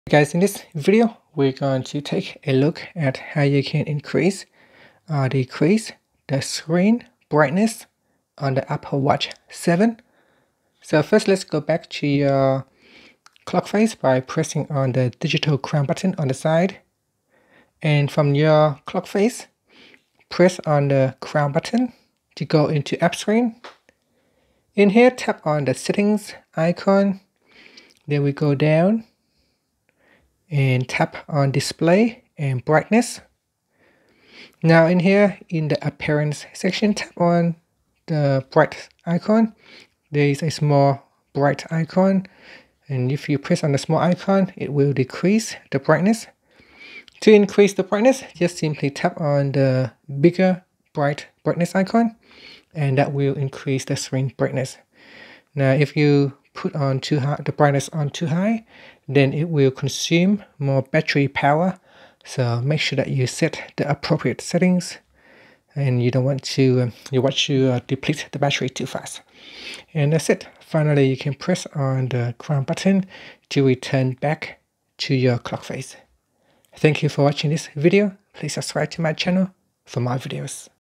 Apple Watch 7: How to Increase/Decrease the Screen Brightness
Learn how you can increase the decrease the screen brightness on the Apple Watch 7.